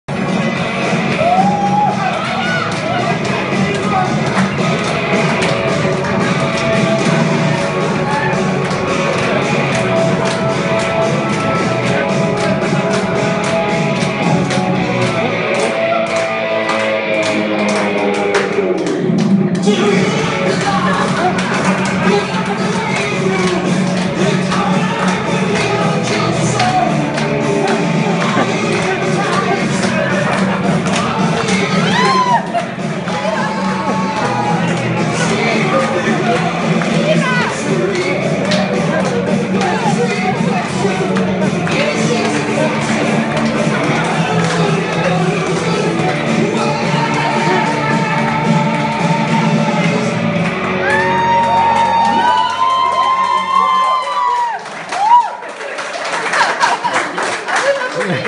Поздравление на День защитника отечества.Ведущий Виктор Баринов
Поздравление на День защитника отечества.Ведущий Виктор Баринов.Сотрудницы компании приготовили много сюрпризов для своих любимых мужчин))Организатор праздника Виктор Баринов. Заказать праздник по телефону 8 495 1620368